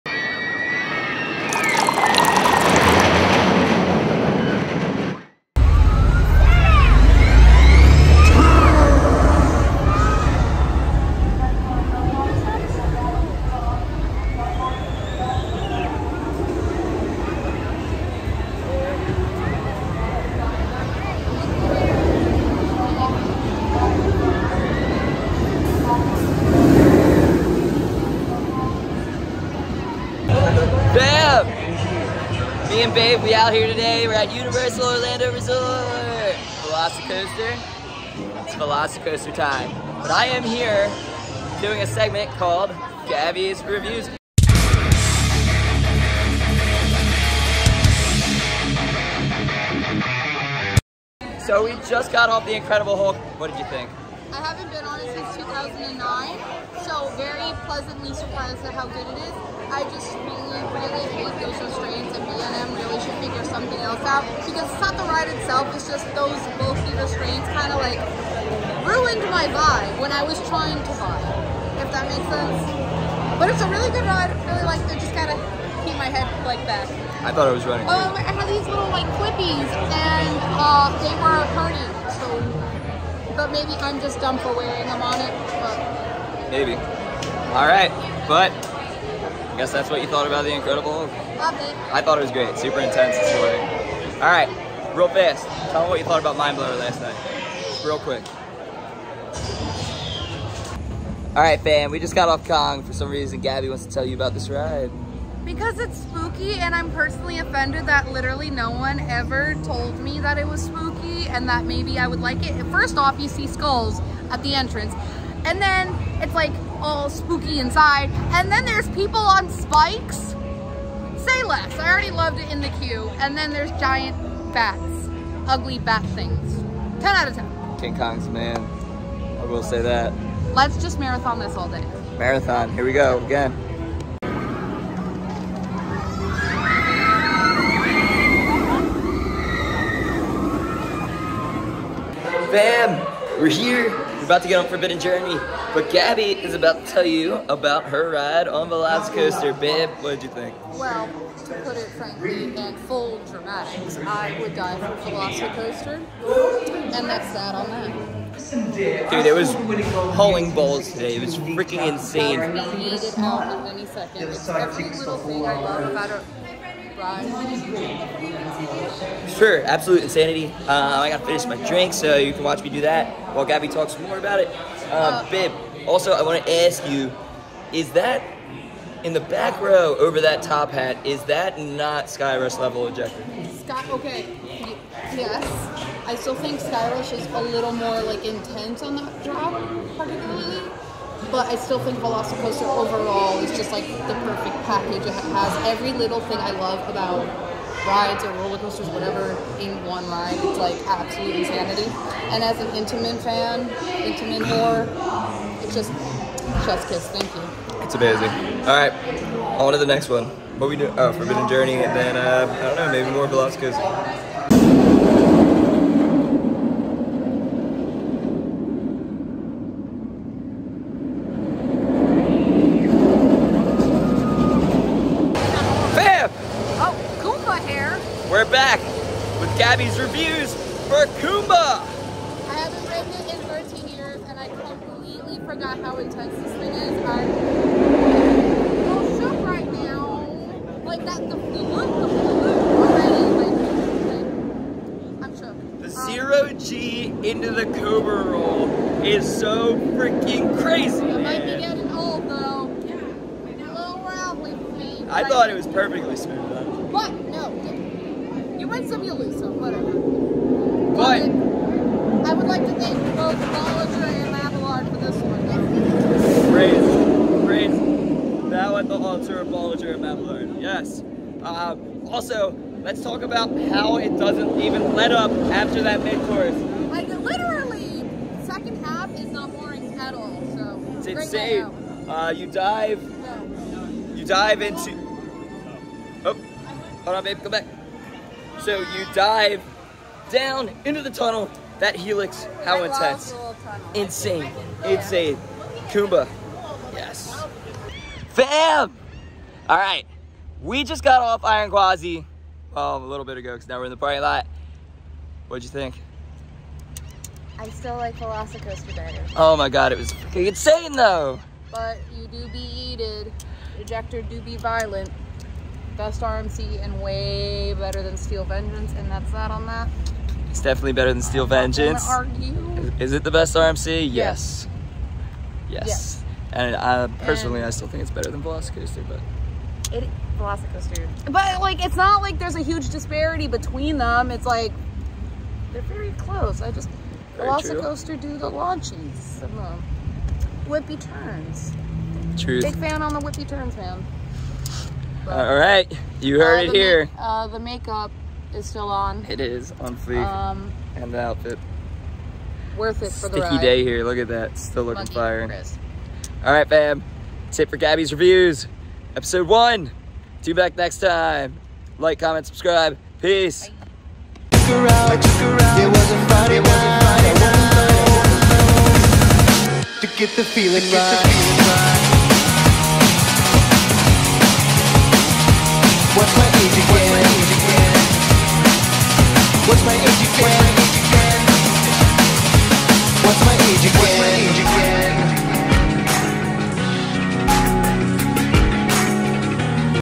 Gabby Reviews Iron Gwazi, Velocicoaster, Kumba, Hulk, and Kong 2022
Gabby and Matt made their way down to Florida in September and Gabby decided to share her thoughts on some of Florida's Favorite roller coasters and rides! check it out and let us know what you think in the comment section below!